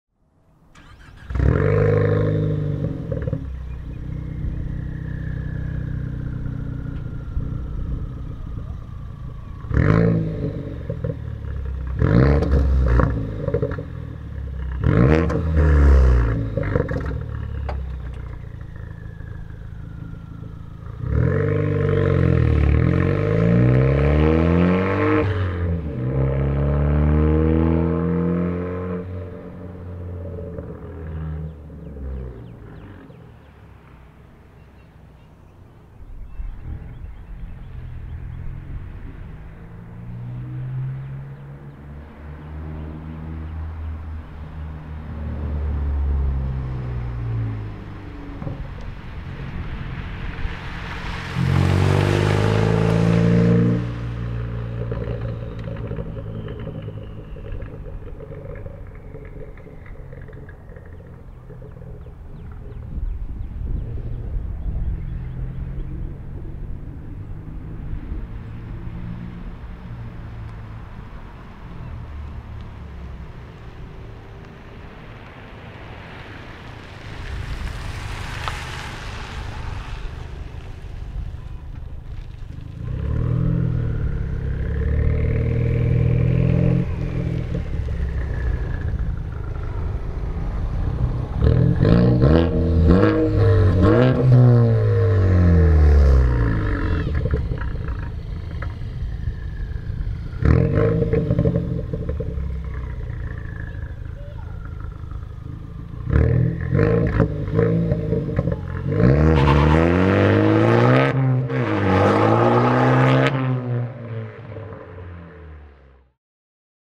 2017 Ford Focus ST Straight Pipe [4K]
Finally got around to filming this video showing my new exhaust.